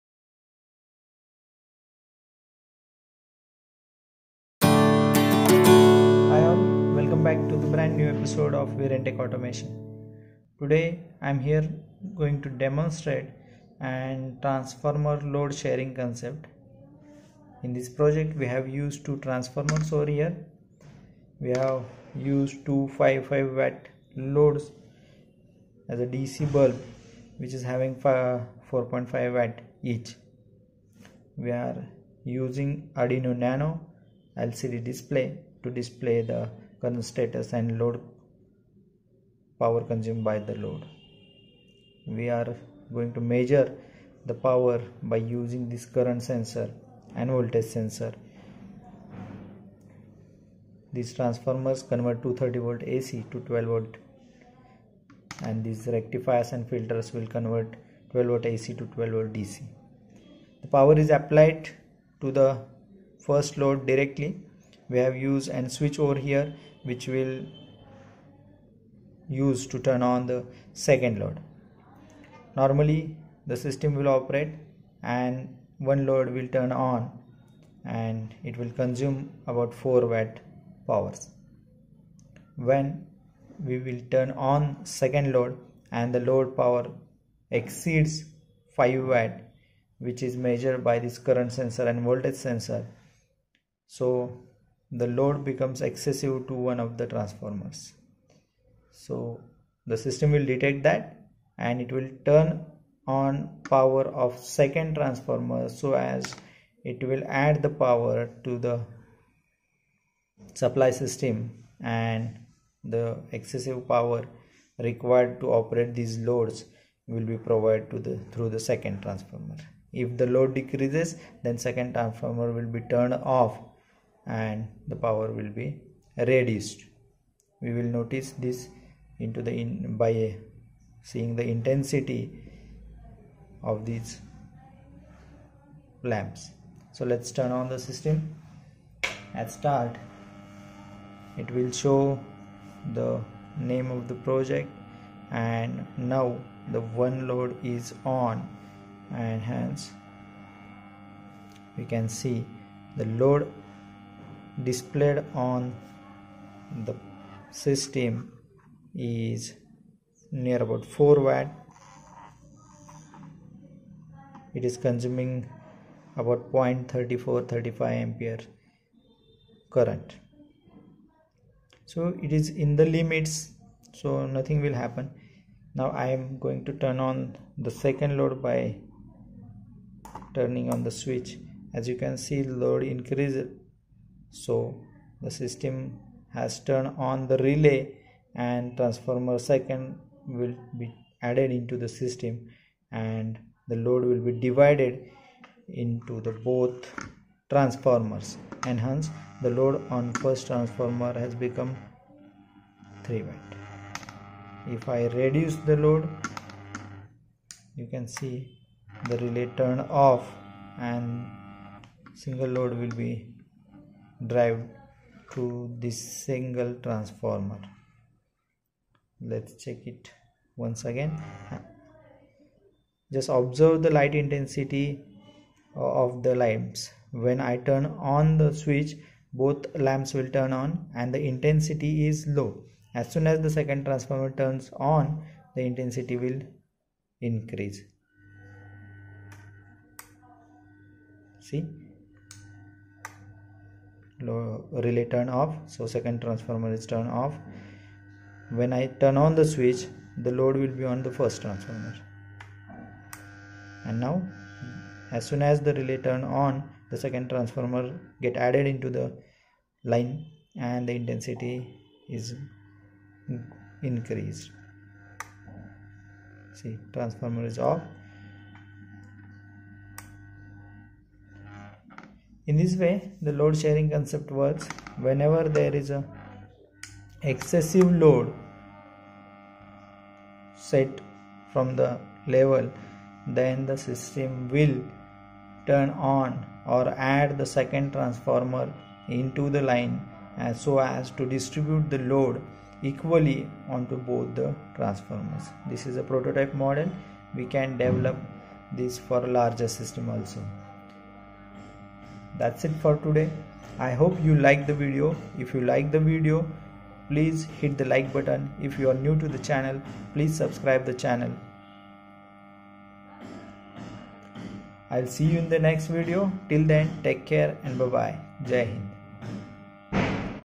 Transformer load sharing system transformer arduino load sharing
Transformer Load Sharing System

User
Transformer load sharing system using arduino

A transformer load sharing system using Arduino can be implemented to distribute the load evenly across multiple transformers. This system can help prevent overloading of individual transformers and improve the overall efficiency and reliability of the power distribution system. Here's a general outline of how such a system can be designed:

Hardware Components:

Transformers: Two or more transformers of equal rating.
Current Sensors: Sensors to measure the load current of each transformer.
Arduino: Microcontroller board such as Arduino Uno or Arduino Mega.
Relay Modules: Relays to switch the transformers on or off.
LCD Display (optional): To display the load status and other information.
Software Implementation:

Connect the current sensors to the Arduino board to measure the load current of each transformer. Make sure to choose current sensors compatible with Arduino (e.g., Hall effect current sensors).
Connect the relay modules to the Arduino. Each relay should control the power supply to a particular transformer.
Write a program in Arduino's programming language (based on C/C++) to read the current sensor values and calculate the load on each transformer.
Implement a load sharing algorithm in the Arduino program. This algorithm will compare the load on each transformer and determine if any adjustments are necessary.
Based on the load sharing algorithm, control the relay modules to switch the transformers on or off. This will distribute the load evenly.
You can also add a display module, such as an LCD, to show the load status and any relevant information.
Load Sharing Algorithm:
The load sharing algorithm will depend on the specific requirements and constraints of your system. Here's a basic example:

Read the current values from all the sensors connected to the transformers.
Calculate the average load by summing up the current values and dividing by the number of transformers.
Compare the load of each transformer with the average load.
If a transformer has a higher load than the average, turn it off by deactivating its relay module.
If a transformer has a lower load than the average, turn it on by activating its relay module.
Periodically repeat the above steps to continuously monitor and adjust the load sharing.